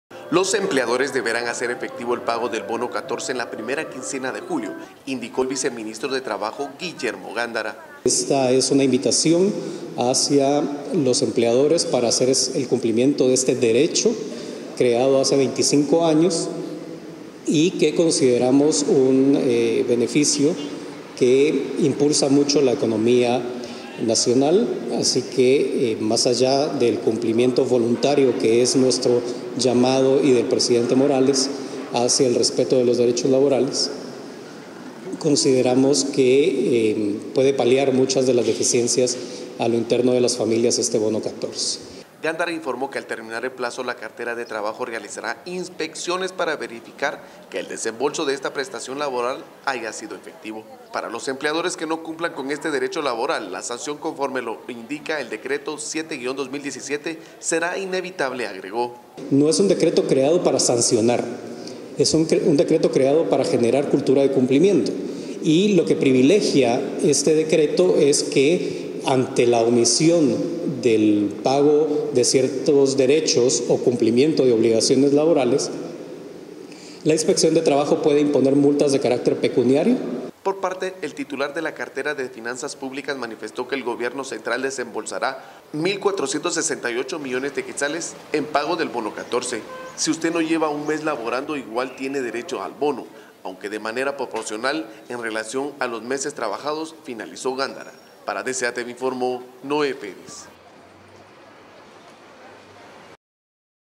Empleadores tienen hasta el 15 de julio para pagar el Bono 14
Los “empleadores deberán hacer efectivo el pago del bono 14 en la primera quincena de julio”, indicó el Viceministro de Trabajo, Guillermo Gándara.

Gándara informó que al terminar el plazo, la cartera de trabajo realizará inspecciones para verificar que el desembolso de esta prestación laborla haya sido efectivo.
Para los empleadores que no cumplan con este derecho laboral, la sanción conforme lo indica el Decreto 7-2017 será  inevitable, agregó.

Por aparte, el titular de la cartera de Finanzas Públicas manifestó que el gobierno central desembolsará Q1 mil 468 millones en pago del Bono 14.

Esta prestación consiste en un salario completo. Si usted no lleva un mes laborando, igualmente tiene derecho al bono, aunque de manera proporcional en relación a los meses trabajados, finalizó Gándara.